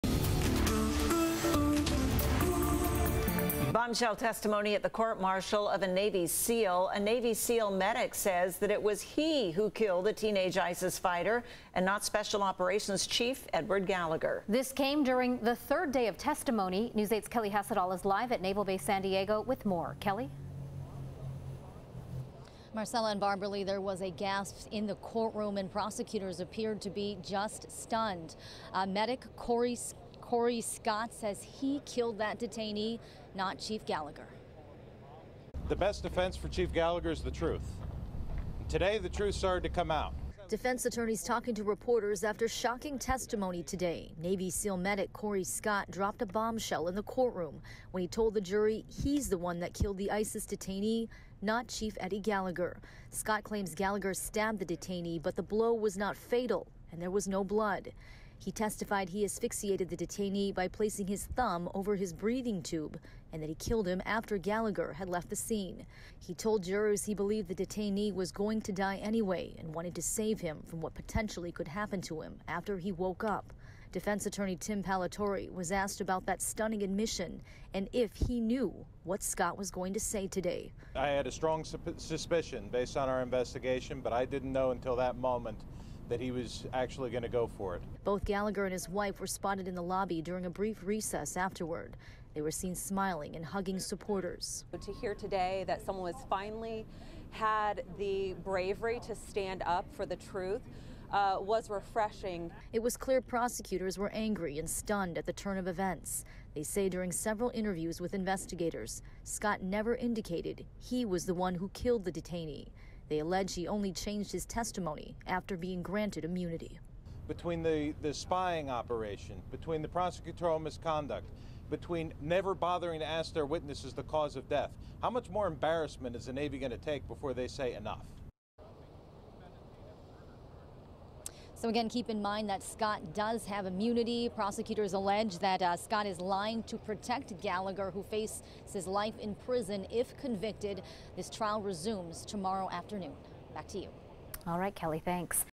Witness at Navy SEAL trial: I killed victim out of mercy
A witness called to testify against a decorated Navy SEAL charged with murder said Thursday that he killed the victim, a bombshell admission he described as an act of mercy for the wounded Islamic State fighter.